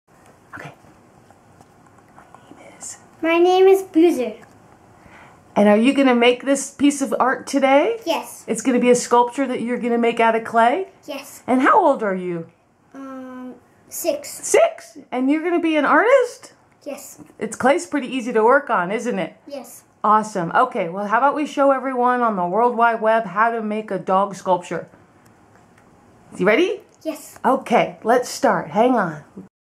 How to Make a Ceramic Dog Portrait Wall Sculpture Part I
Whacky and whimsical ceramic dog portrait wall sculpture techniques demonstrated by an adorable 6 year old boy to show how easy it is create a piece of art.